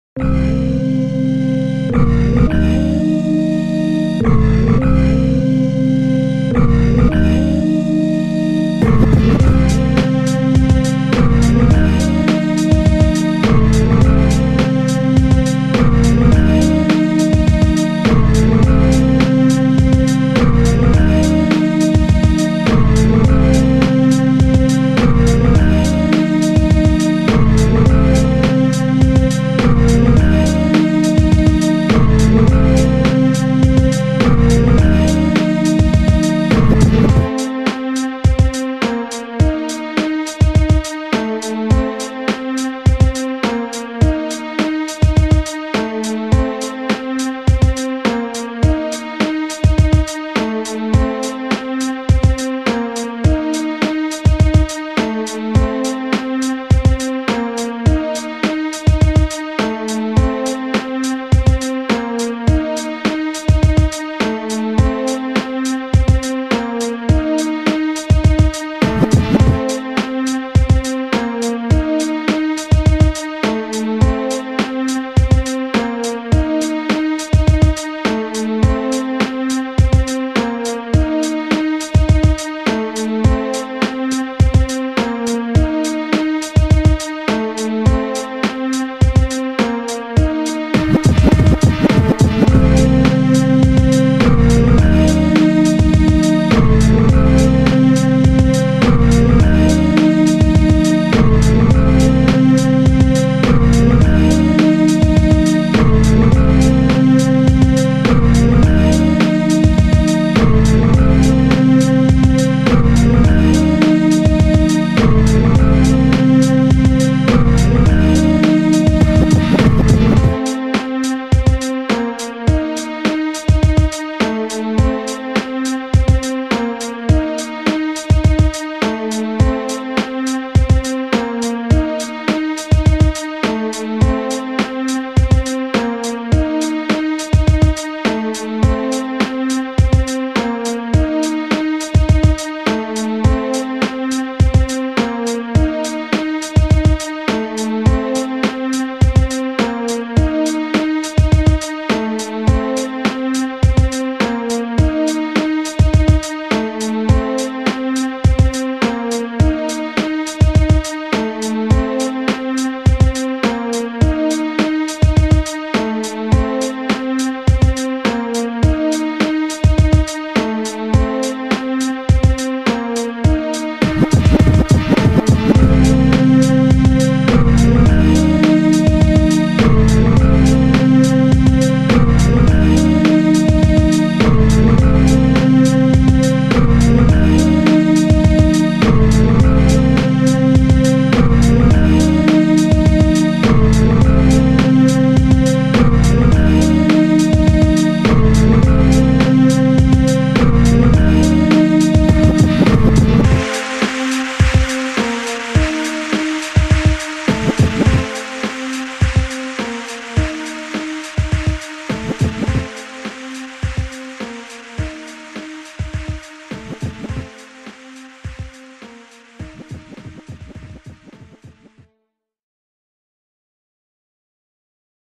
Dj Premier Inspired Beat
This beat is inspired by the one and only Dj Premier! Watch out for more beats to come!
if you wanna hop on this beat! inbox me!